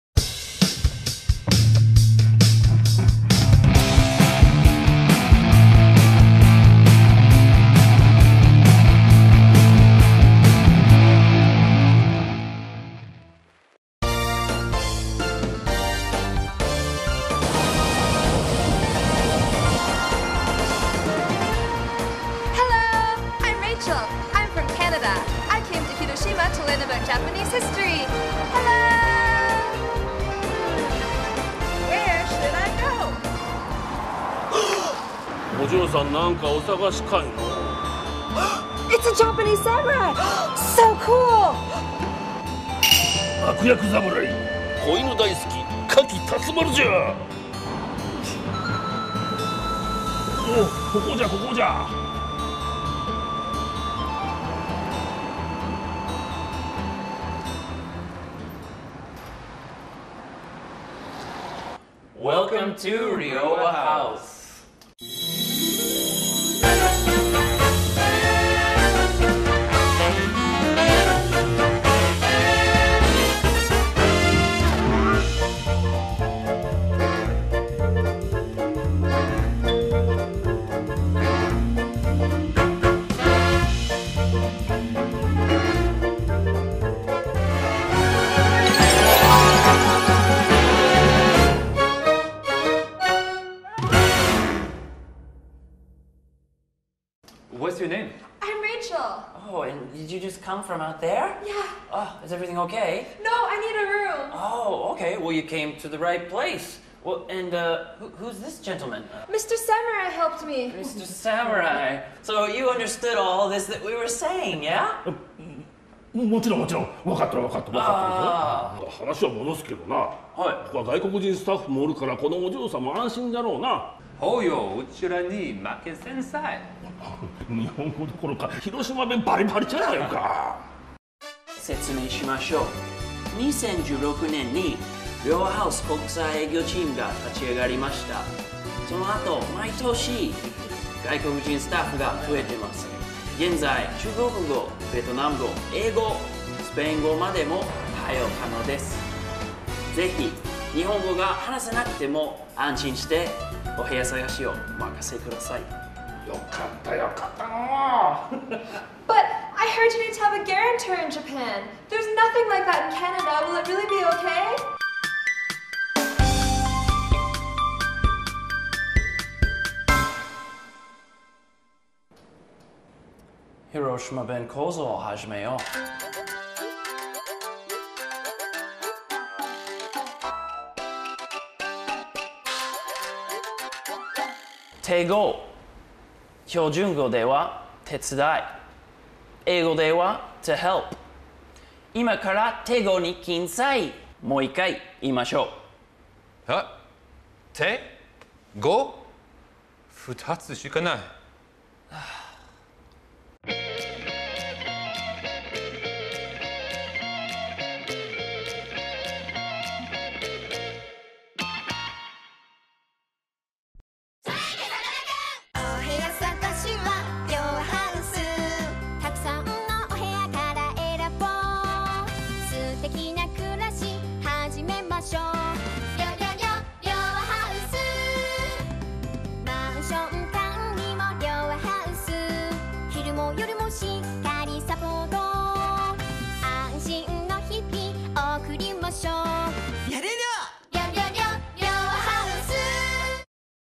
Japanese Samurai Real Estate Breakdown! サムライと困ったお嬢さんが不動産に来店する
日本語は下

Introducing a new series with new characters. We layout the Japanese real estate system in the south part of Hiroshima with a local Hiroshima Samurai and Hiroshima newcomer, Rachel!

新しいシリーズ！広島の南エリアで困っていたレイチェルさんが広島のローカルサムライに出会います！そこで二人で良和ハウス南店でお部屋探しをスタートします